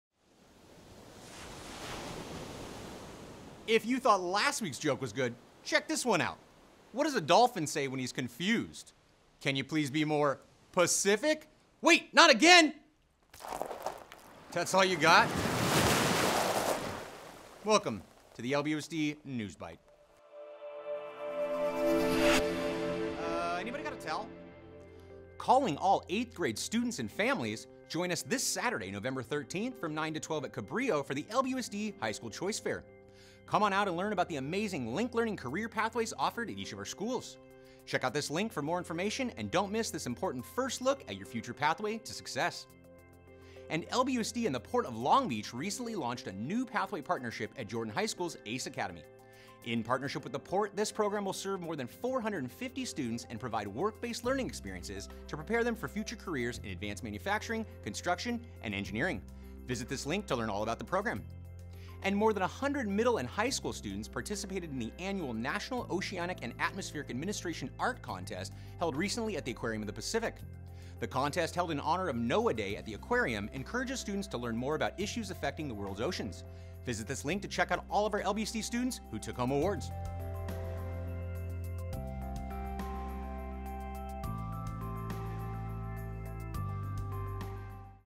LBUSD NewsByte - November 10th, 2021
On Today's Episode...
New partnership between LBUSD and The Port of Long Beach, at Jordan High School
2021 High School Choice Fair at Cabrillo High School
NOAA Day Student Art Contest at the Aquarium of the Pacific